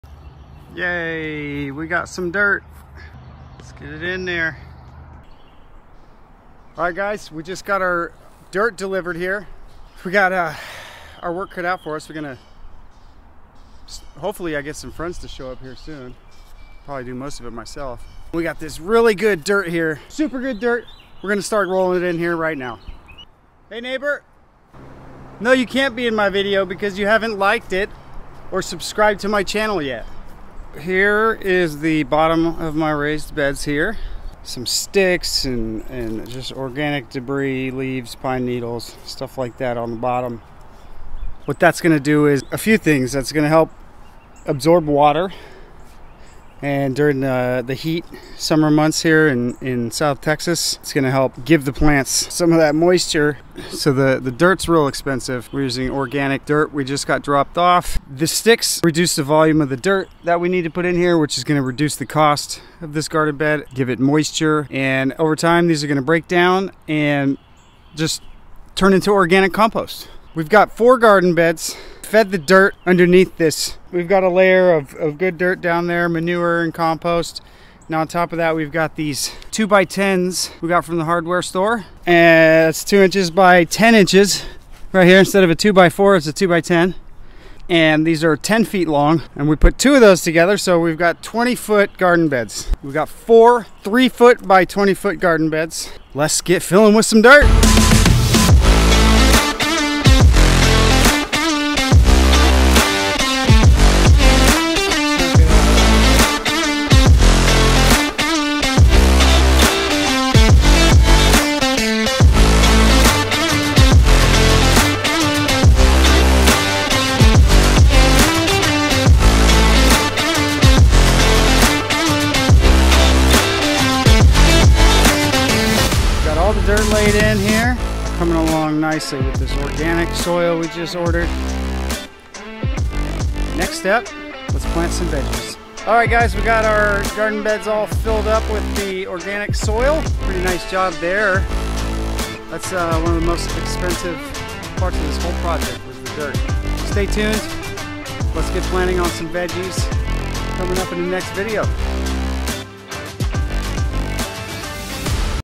The LAZY Gardener's Guide to a Thriving Organic Raised Bed (with Soil Delivery & Planting Tips!)
Creating The Ultimate Organic Raised Garden Bed: Tips For Perfect Soil Composition!

Whether you're interested in growing vegetables, herbs, or flowers, this video has something for everyone. Join us as we unlock the secrets to creating vibrant, thriving organic garden beds that will yield bountiful harvests for years.

00:00 Introduction
00:30 Bed Composition
01:18 Bed Design
01:37 Hauling Dirt
02:06 Result
02:16 Outro